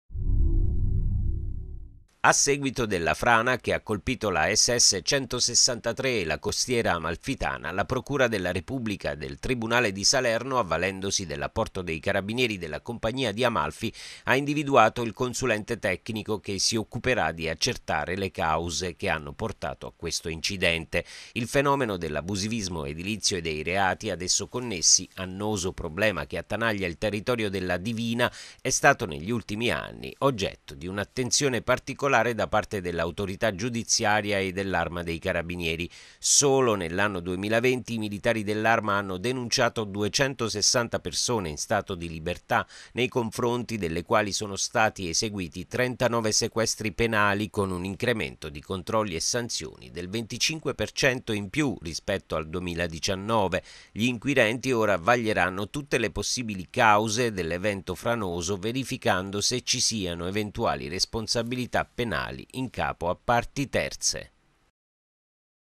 Costiera, Frana ad Amalfi, Primo sopralluogo del consulente della procura.
A seguito della frana che ha colpito la SS163 e la Costiera Amalfitana, la Procura della Repubblica del Tribunale di Salerno, avvalendosi dell'apporto dei Carabinieri della Compagnia di Amalfi, ha individuato il consulente tecnico, che si occuperà di accertare le cause che hanno portato a questo incidente.